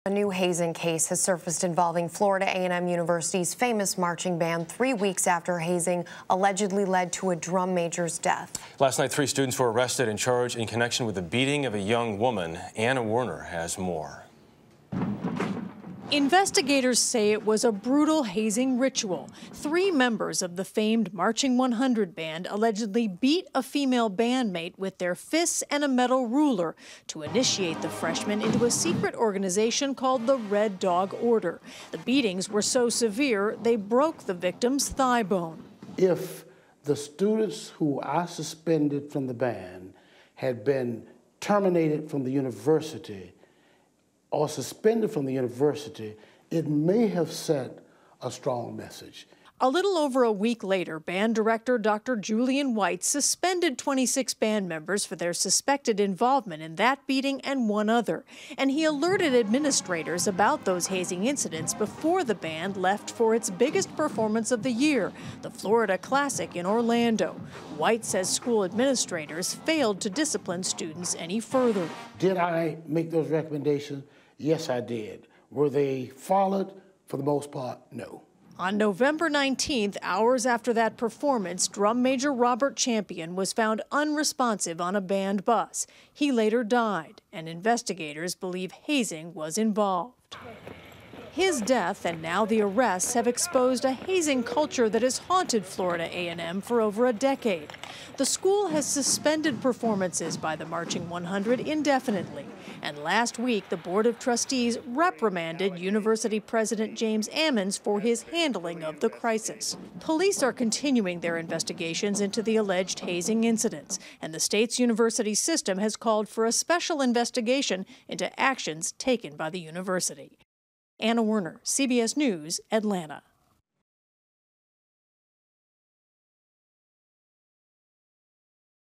Hazing at A&M - 3 arrested in new case
A new hazing case has surfaced involving Florida A&M University's famed "Marching 100" band. Three students were arrested and charged in connection with the alleged beating of a freshman woman. CBS News correspondent Anna Werner reports.